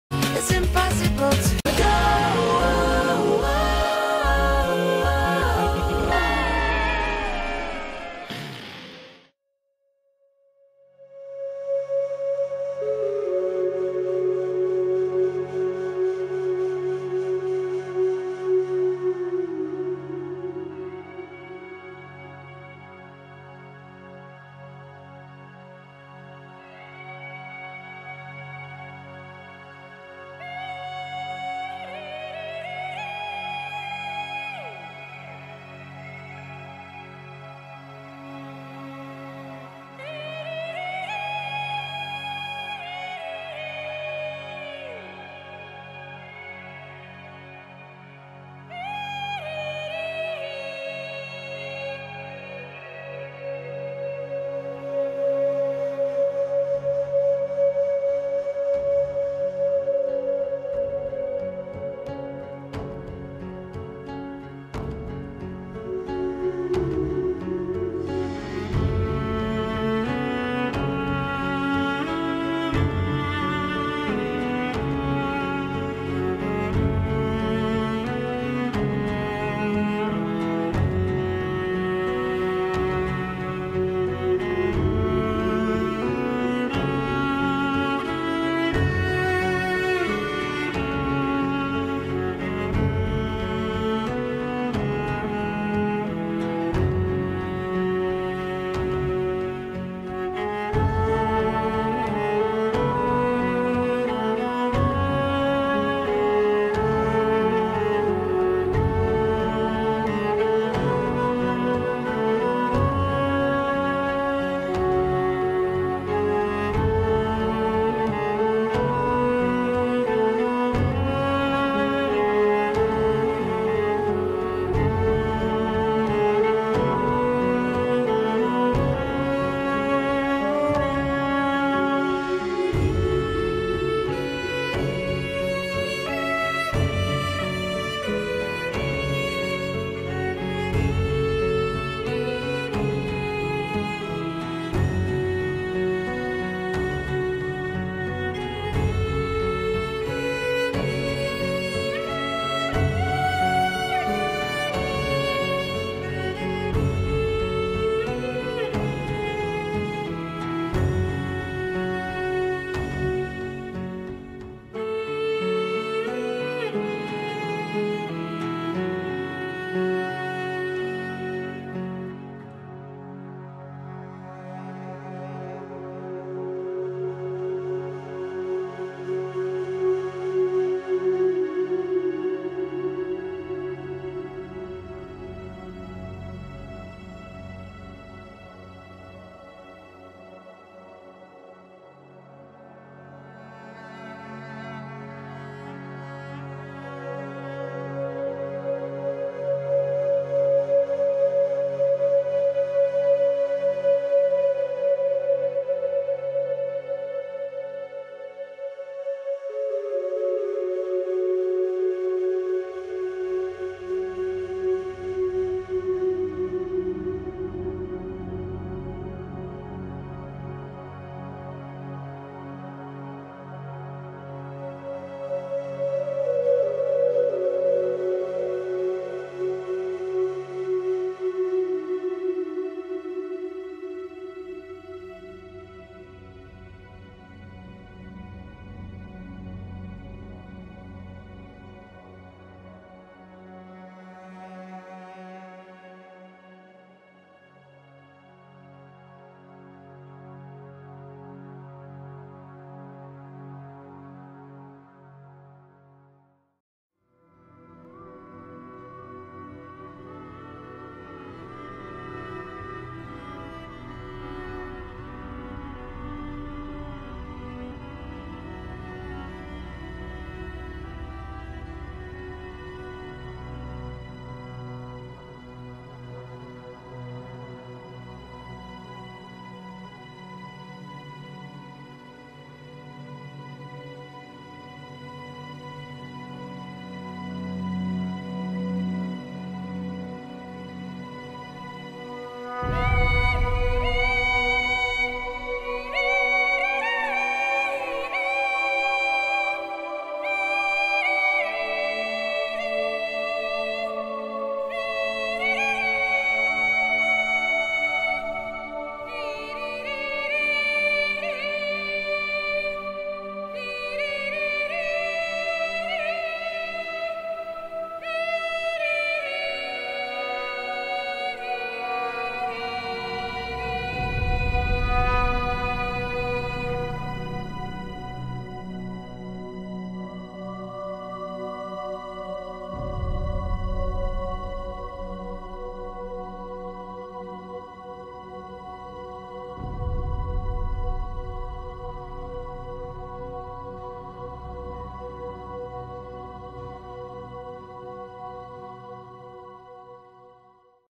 Brothers: A Tale of Two Sons [Speed-Paint]
Brothers: A Tale of Two Sons is a downloadable story-driven adventure video game developed by Starbreeze Studios and released on August 7th, 2013. It was being developed in collaboration with the Swedish award-winning film director Josef Fares.